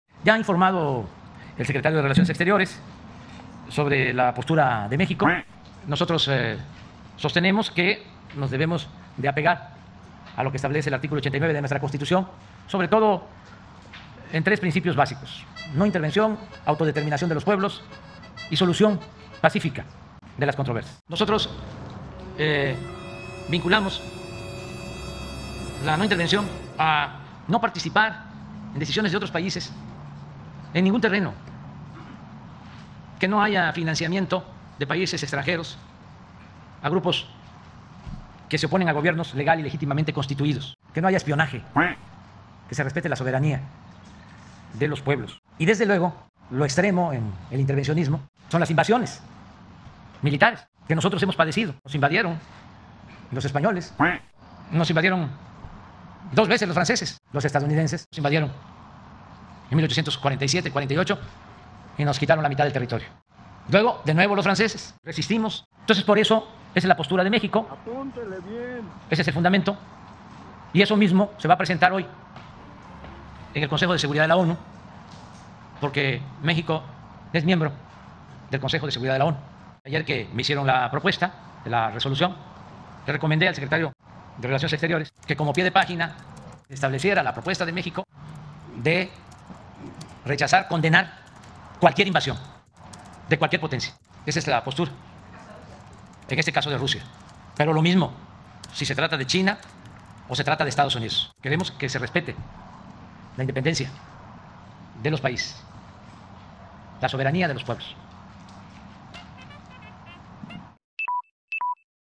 México hará un posicionamiento ante la ONU para condenar la invasión de Rusia a Ucrania
Condena las invasiones militares, ya sea la de Rusia, la de China o la de Estados Unidos, comentó que México hoy hará ese posicionamiento ante ONU 🇺🇳 🕊🇺🇦